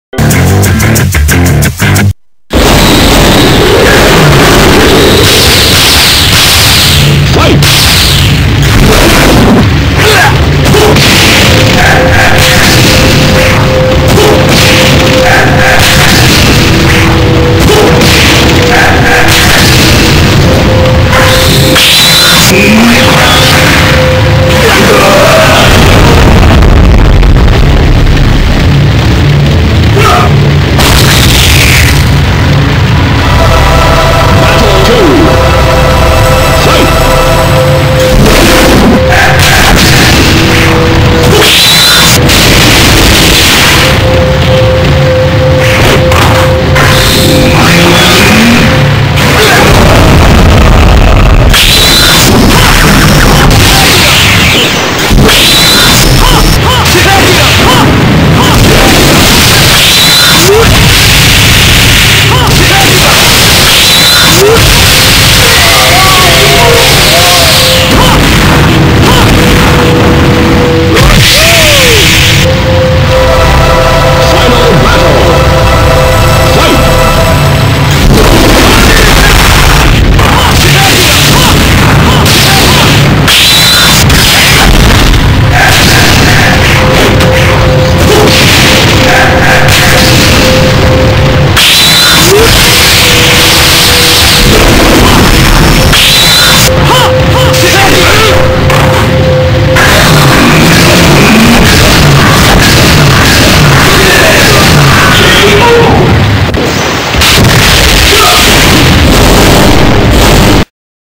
Taking down Perfect Storm, the second time!
Just to rub it in, I beat him again. Could've done better, with some better timing, but he still got beat.